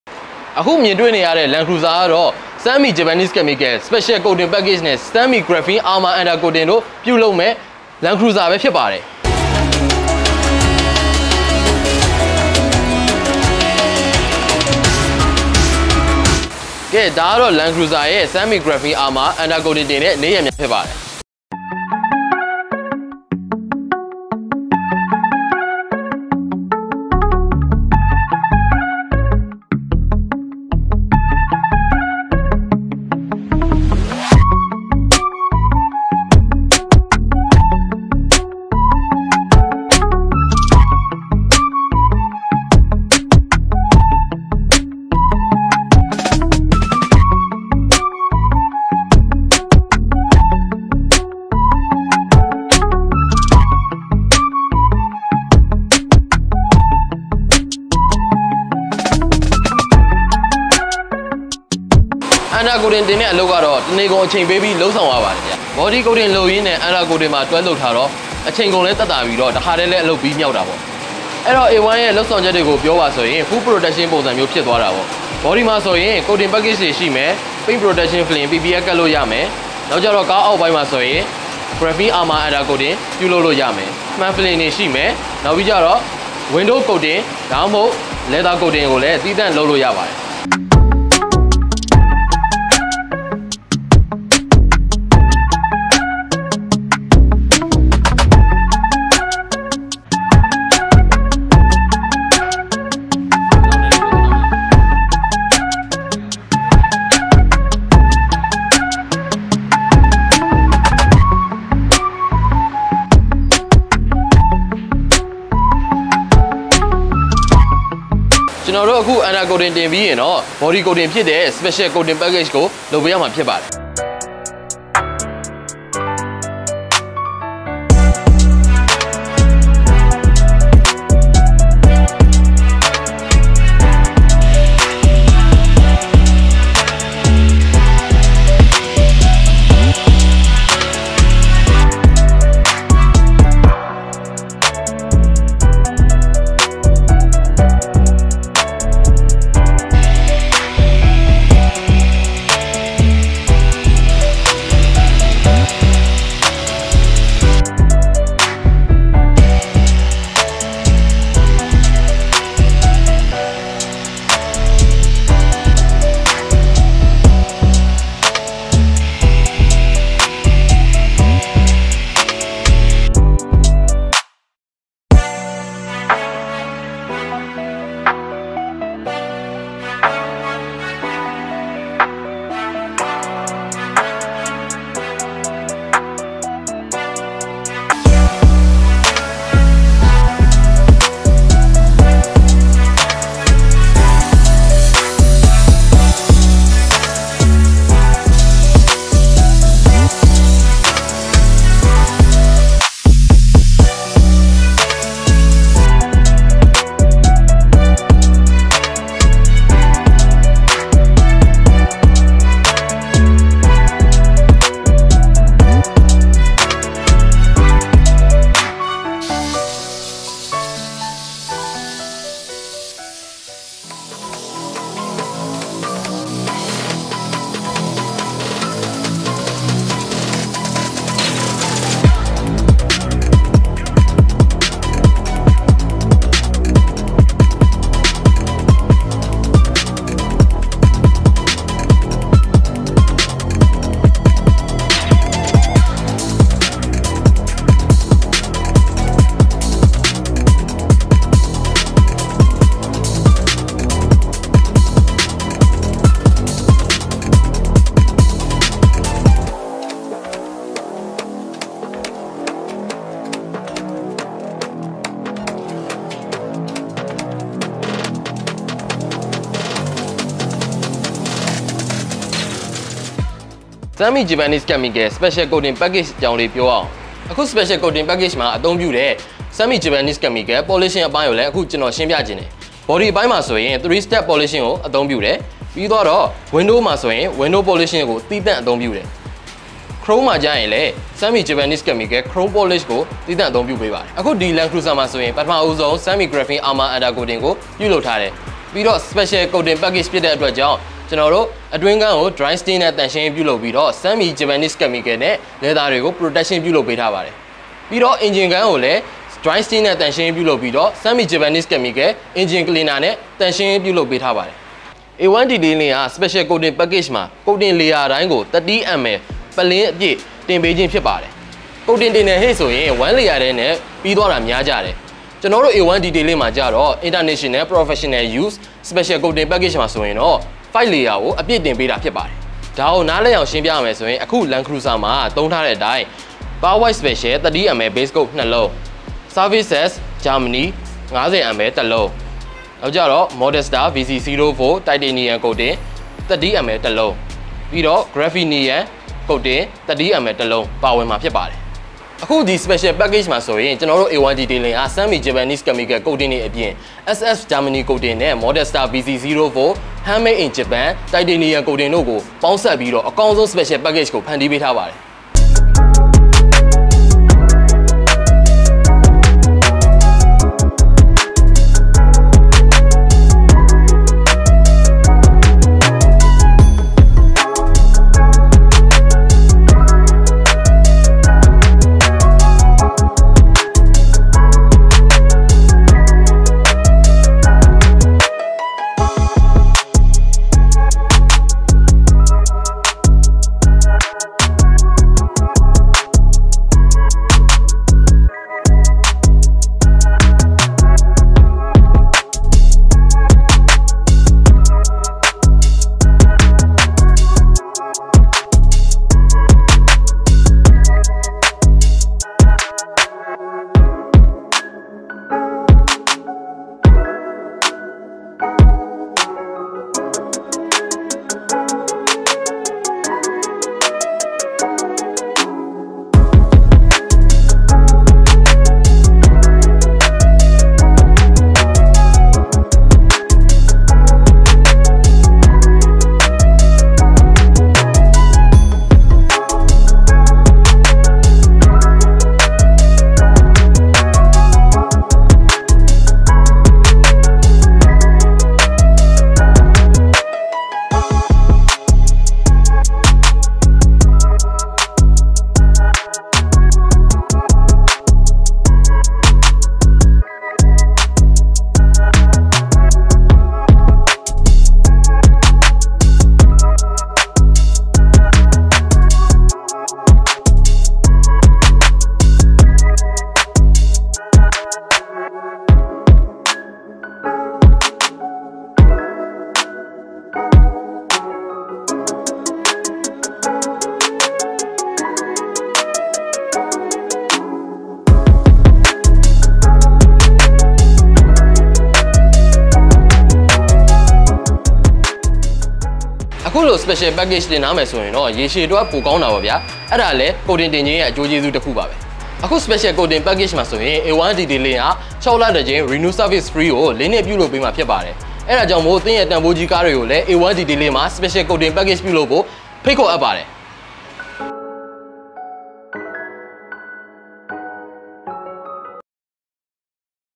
TOYOTA LANDCRUISER - SAMI GRAPHENE UNDER COATING & GRAPHENE COATING
WELCOME BACK!!!

Landcruiser is graphene coated undercarriage and body. It protects the under body from rusting in future. The paint is fully coated in SAMI GRAPHENE SPECIAL COATING PACKAGE. In this package, 2 layers of SAMI Graphene Pearl White Special Coating (30ml) , Modesta BC-04 Titanium Coating , ServFaces Coat Ultima and SAMI DIAMOND CANDY GLOSS GRAPHENIUM (30 ml ) are used to coat the body. Great gloss and great hydrophobic effect.

We give free renew services per 6 MONTHS to 4 YEARS.

                   1045A , Near Zarga St, on Waizayantar Rd. South Oakalarpa. Ygn.
                    Myanmar.

Credits: all music in this video are not our own.